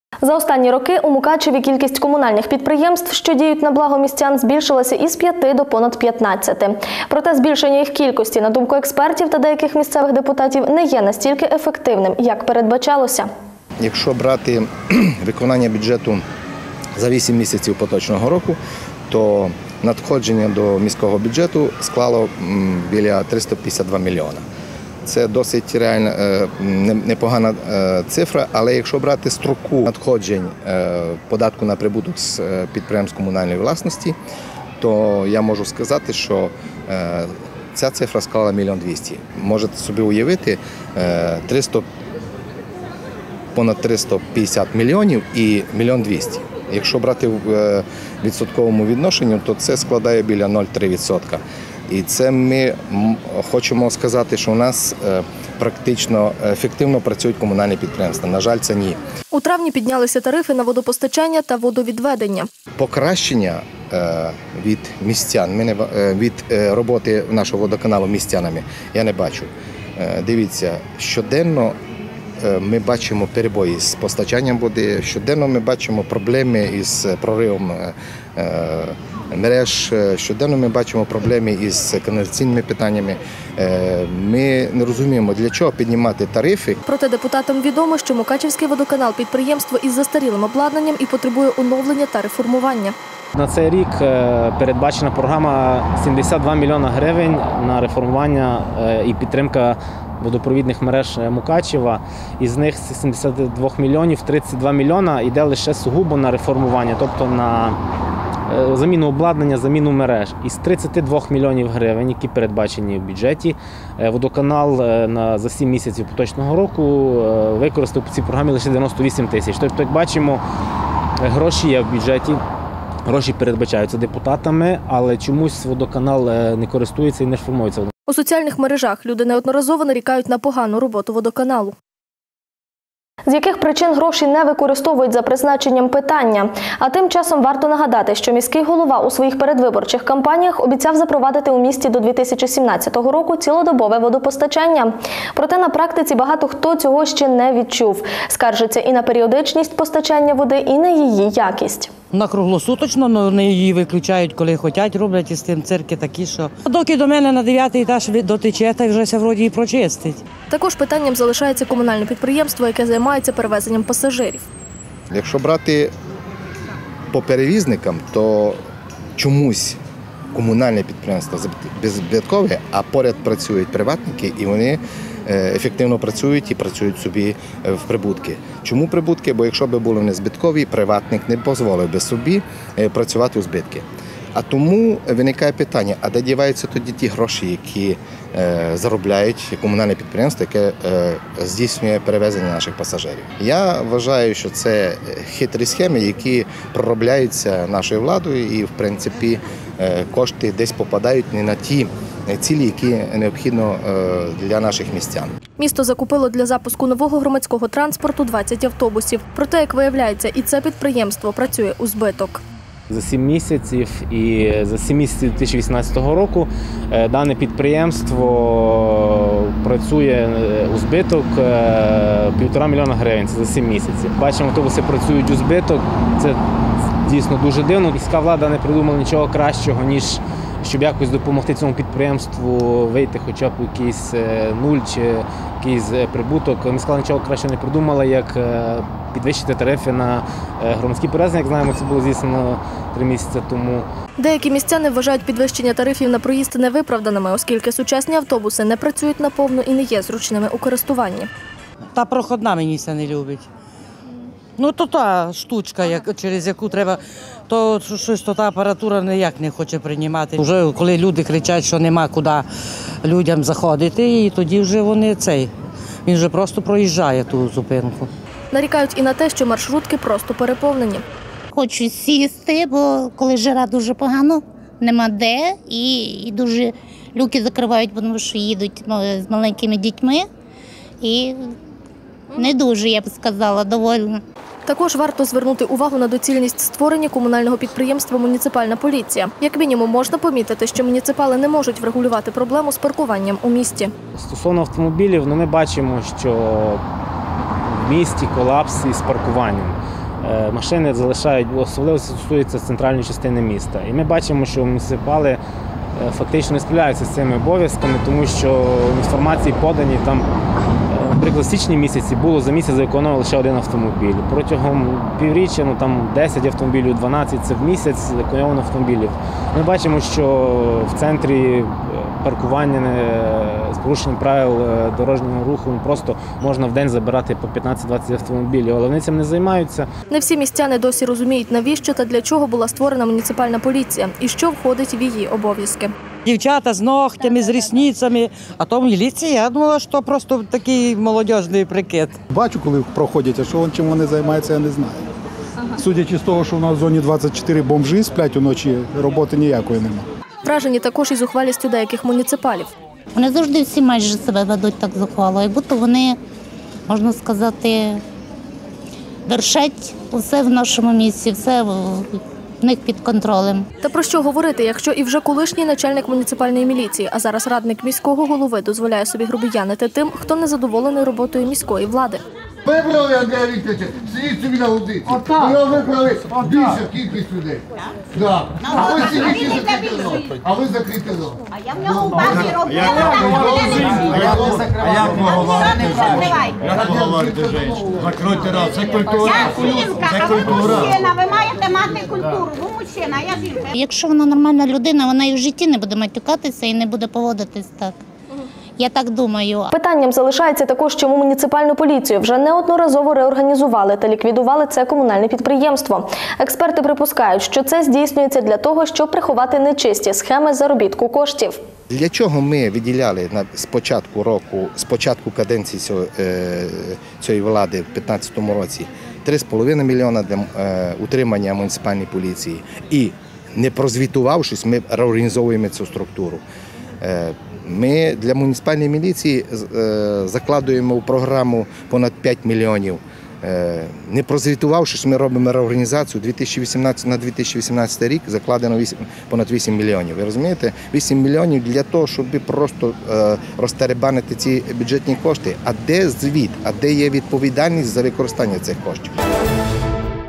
Збиткові комунальні підприємства
За останні роки в Мукачеві кількість комунальних підприємств, що діють на благо містян, збільшилася із 5 до понад 15. Проте збільшення їх кількості, на думку експертів та деяких місцевих депутатів, не є настільки прибутковим, як передбачалося.  Зокрема, мова іде про мукачівський водоканал, пасажирські перевезення та муніципальну поліцію.